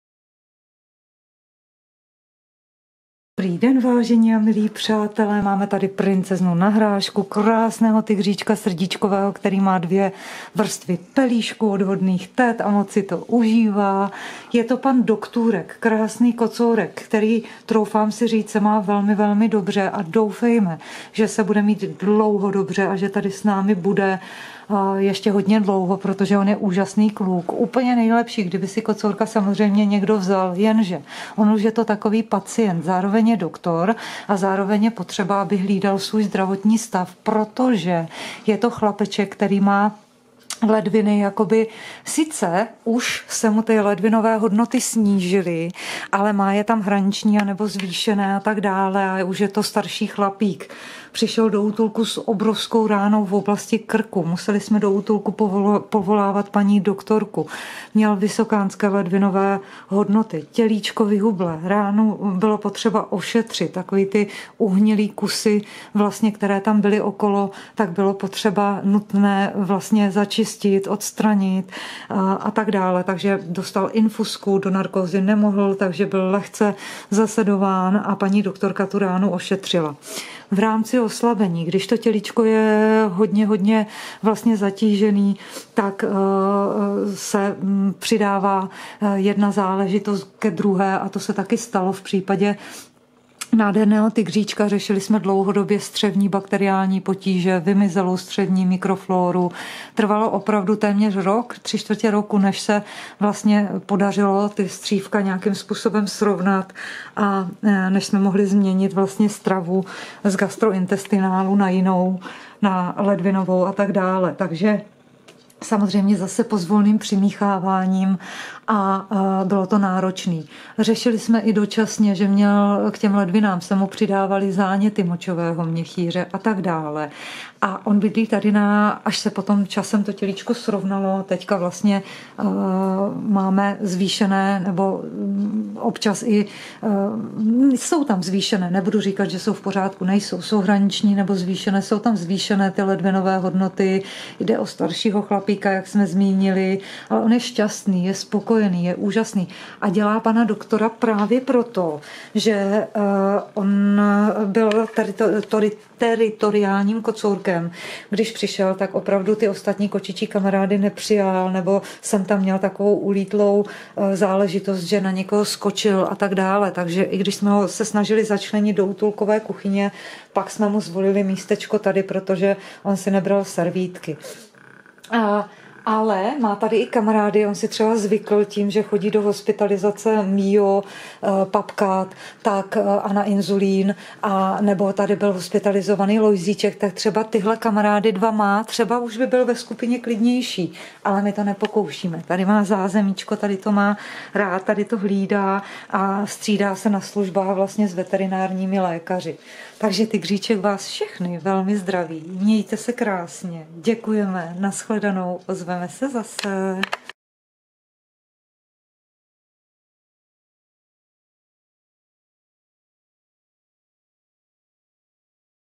Tygříček (19.4.2022)
❤️ Velikonoční nadílka pro Tygříčka Srdíčkového ❤️ Vážení a milí přátelé, dnes má Tygříčkův svůj velký den, slaví totiž útulkové Velikonoce a to je velká paráda. Koukněte na kocourkovo video z útulkové vyšetřovny. MVDr. Tygříček Srdíčkový pochází z Kladenska, kde jej našli moc hodní lidé a poskytli mu první pomoc. Do útulku přišel s obrovskou ránou v oblasti krku, vyhublý, dehydrovaný, se zvýšenými ledvinovými hodnotami a dalšími patáliemi. Řešili jsme močové cesty a výrazné střevní bakteriální potíže. Po delším čase jsme se zbavili průjmu i potíží s močovými cestami, to vše souviselo s dramatickým vstupním stavem kocourka při příjmu. Ledvinové hodnoty jsou proměnlivé, ale rozhodně únosné. Kocourek po čase velmi prokoukl, zmohutněl a před časem se dočkal i kastrace. Z Tygříčka se stal pan doktor, který střeží útulkovou vyšetřovnu a velmi rád ordinuje, mnohdy na úkor přesčasů. Kdysi bydlel ve výběhu trojce, tu však musel opustit, protože jsme výběh potřebovali pro FIV pozitivní kamarády. A nyní pojďme k nadílce. Doktůrek Tygříček si bude k Velikonocům přát dva hlavní potřebné dárečky. Přispívat chlapíkovi můžete rovněž na důležitou a nezbytně nutnou veterinární péči v hodnotě 200Kč nebo 400Kč a za tento příspěvek obdržíte nádherný certifikát, zároveň je možné nasypat korunky do bříška = plnit mističky. Všechny dárečky naleznete v srdíčkovém albíčku. Kočičkám může jakýkoli dárce přispívat v průběhu nadílky neomezeně. Všem přejeme krásné Velikonoce, hodně zdraví a velmi hodným dárcům děkujeme za případnou podporu a pomoc ❤